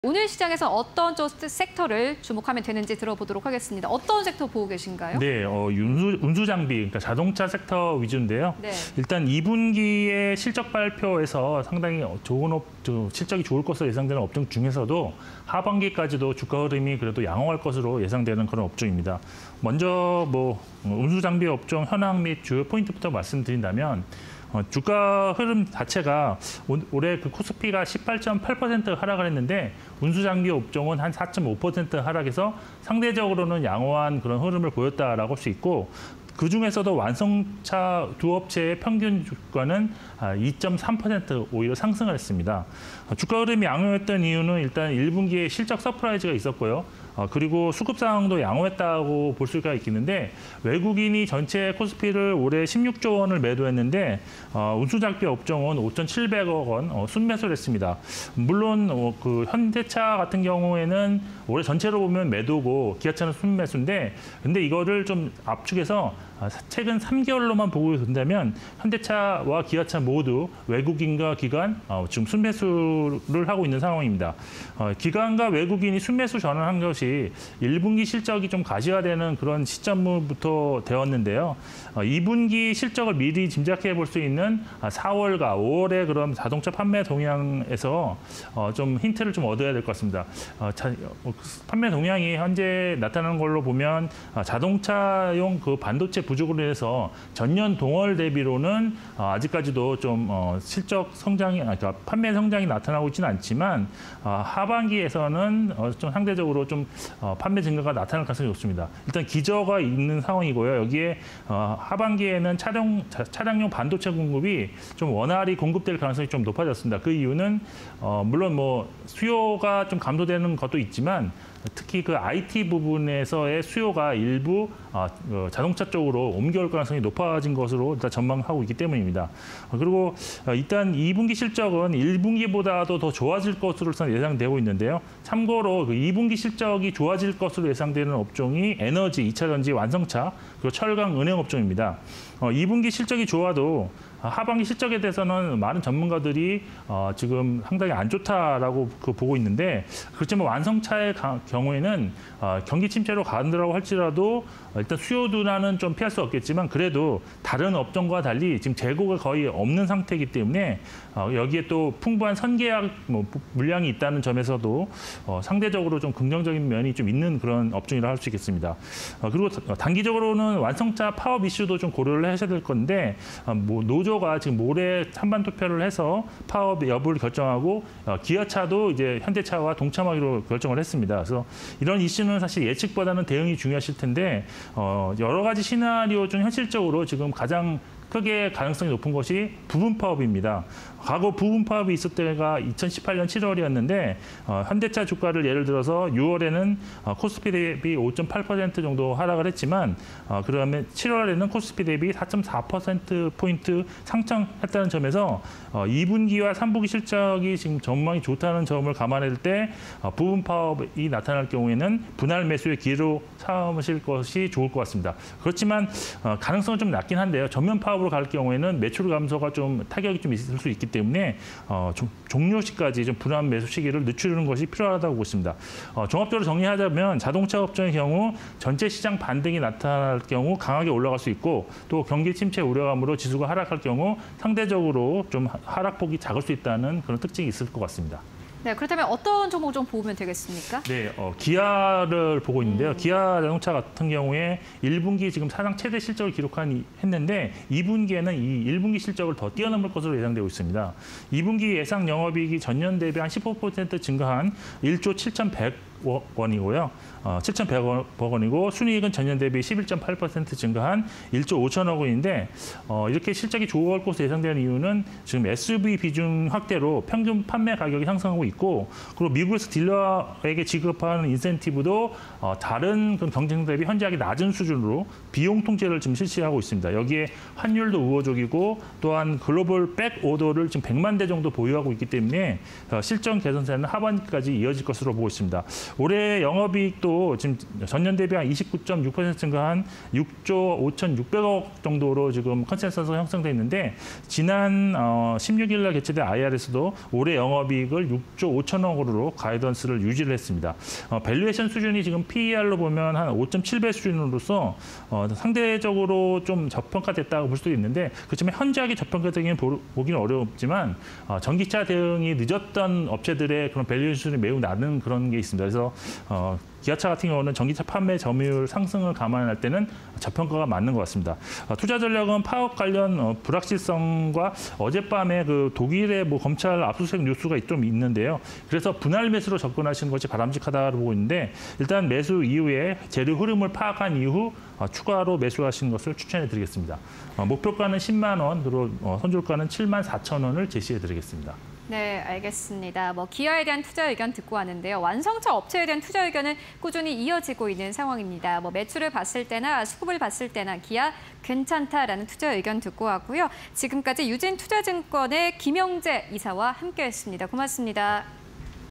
기아(000270)... 자동차 업종_시초가에 이 종목 (20220629)
자동차 업종
- 올해 코스피 하락률(-18.8%) 대비 양호한 흐름
- 외국인, 올해 코스피 16조 원 순매도 속 운수장비 업종 5,700억 원 순매수
- 4·5월 자동차 판매, 반도체 부족에 판매 증가세 둔화
- 2분기 호실적 예상 업종: 에너지·2차전지·완성차·철강·은행
- 경기 침체 시 수요 둔화 부정적이나 재고 없는 점 긍정적 평가
- 완성차 업계 파업 관련 불확실성 존재

기아(000270)
- 2분기 영업이익 1.71조 원… 전년대비 15%↑
- 실적 개선 지속 전망
- 올해 영업이익 6.56조 원… 전년대비 29.6%↑
- 현재 주가 PER 5.7배 수준
- 파업 관련 불확실성 존재, 분할 매수 접근 필요